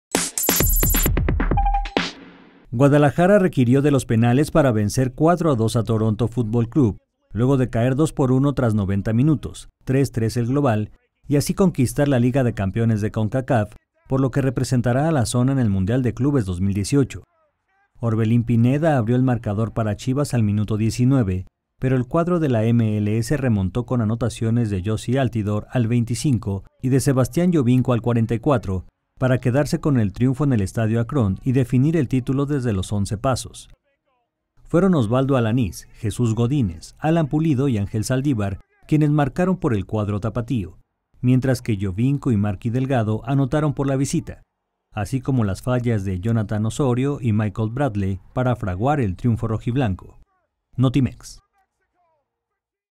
Chivas, campeón de Concachampions 2018; va a Mundial de Clubes
Guadalajara requirió de los penales para vencer 4-2 a Toronto FC, luego de caer 2-1 tras 90 minutos (3-3 global) y así conquistar la Liga de Campeones de Concacaf, por lo que representará a la zona en el Mundial de Clubes 2018.